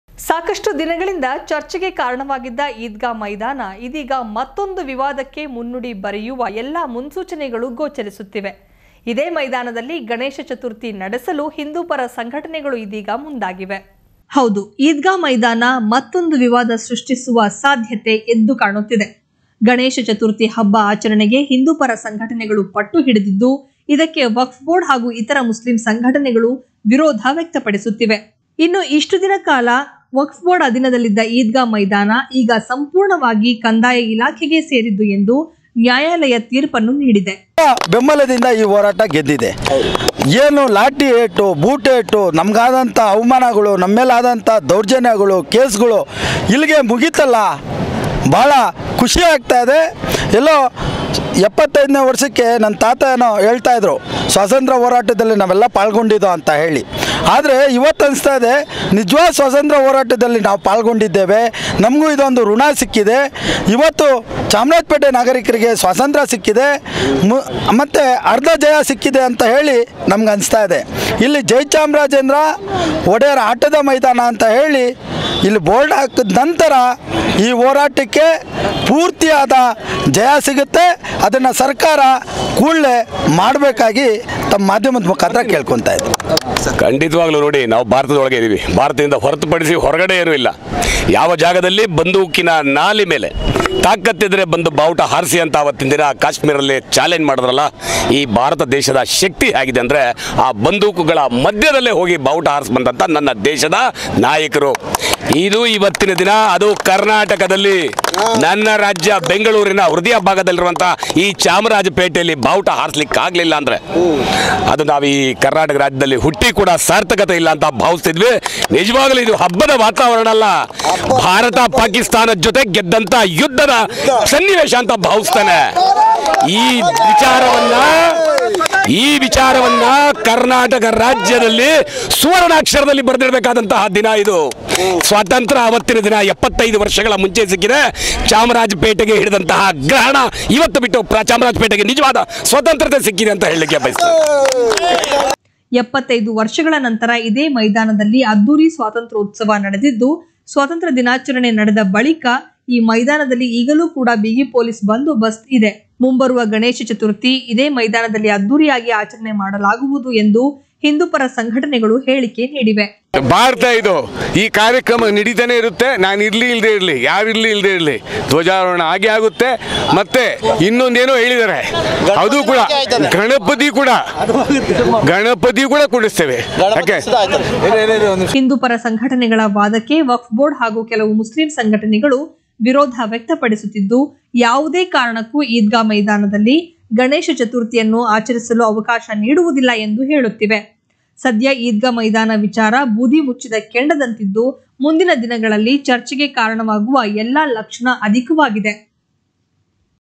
ಚತುರ್ಥಿ ನಡೆಸಲು ಮುಂದಾದ ಹಿಂದೂ ಸಂಘಟನೆಗಳು | ಈದ್ಗ ಮೈದಾನ ಮತ್ತೊಂದು ವಿವಾದ | Samagra News
ಈದ್ಗ ಮೈದಾನ ಮತ್ತೊಂದು ವಿವಾದ
ಚತುರ್ಥಿ ನಡೆಸಲು ಮುಂದಾದ ಹಿಂದೂ ಸಂಘಟನೆಗಳು
ಮುಸ್ಲಿಂ ಸಂಘಟನೆಗಳು ವಿರೋಧ ವ್ಯಕ್ತ